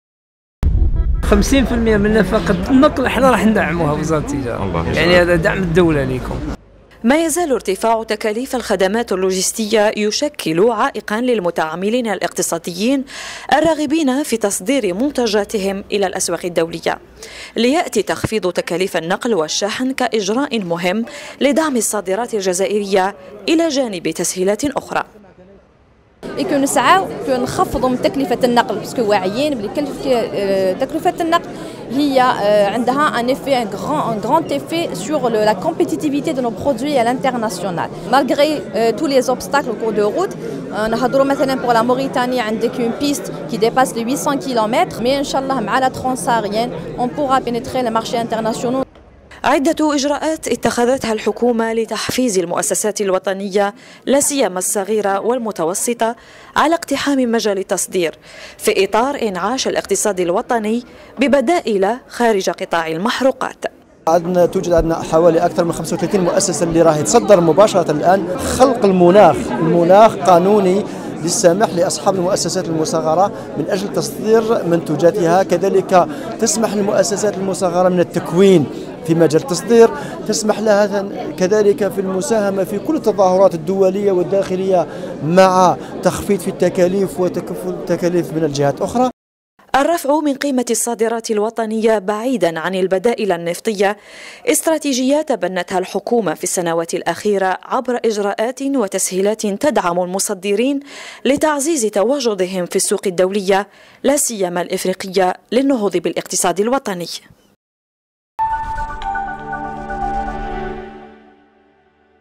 تخفيض تكاليف النقل و اللوجستيك ..ضرورة للدفع بحجم الصادرات
الجزائر (وأج)- تطوير الخدمات اللوجستية يشكل  أحد المطالب الأساسية  للمتعاملين الاقتصاديين بشكل يسمح لهم من تصدير منتجاتهم الى الاسواق الدولية و هو في نقس الوقت ماتسعى الحكومة لتجسيده  في اطار مخطط انعاش الاقتصاد الوطني .